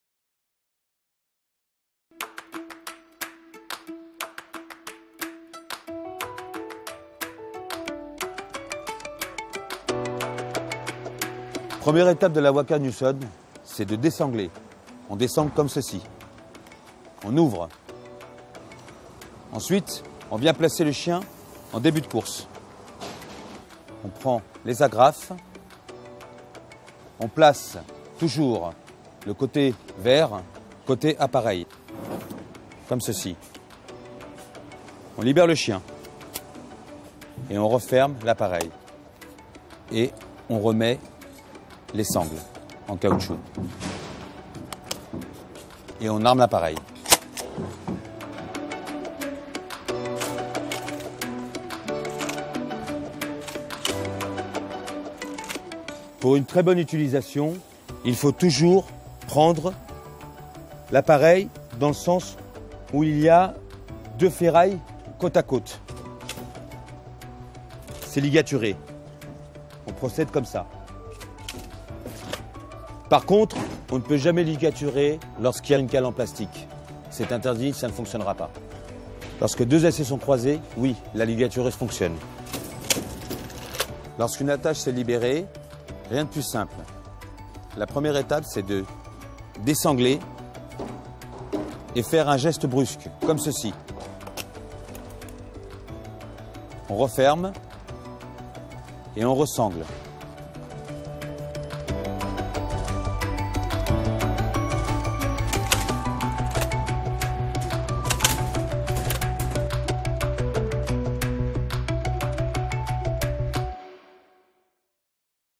ByTubes - Utilisation de la ligatureuse Wacker Neuson
Pour utiliser la ligatureuse Wacker Neuson de manière
précise et méthodique, suivez les différentes étapes.
1. Première étape de la Wacker Neuson c’est de désangler.
2. Placer le chien en début de course.
3. Prendre les agrafes et toujours placer le côté vert côté appareil, libérer
le chien et refermer l’appareil.
4. Remettre les sangles en caoutchouc et armer l’appareil.
5. Pour une très bonne utilisation, il faut toujours prendre l’appareil dans
le sens où il y a deux ferrailles côte à côte.
Remarque : on ne peut jamais ligaturer lorsqu’il y a une cale en plastique,
c’est interdit ça ne fonctionnera pas.
Lorsque deux aciers sont croisés, la ligatureuse fonctionne.

Lors de ce tournage, les équipements de protection individuels portés par les intervenants étaient conformes à la politique de l’Entreprise. Celle-ci a évolué en date du 1er janvier 2019.